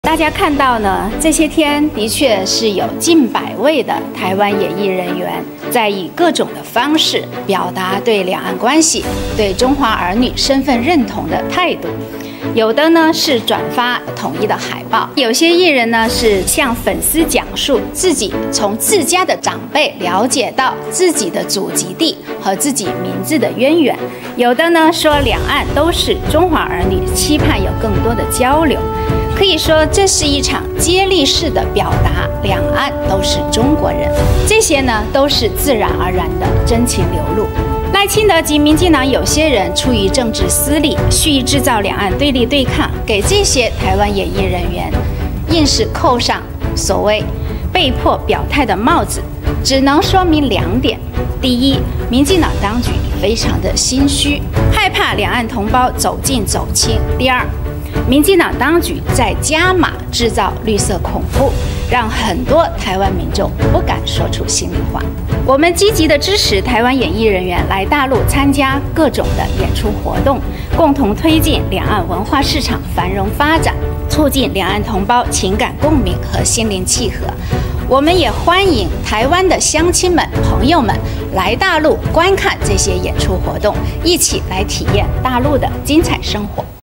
近百位台灣藝人表態支持統一 國台辦回應：是身份認同的真情流露！
近百位台灣藝人表態支持統一 國台辦回應：是身份認同的真情流露！國台辦發布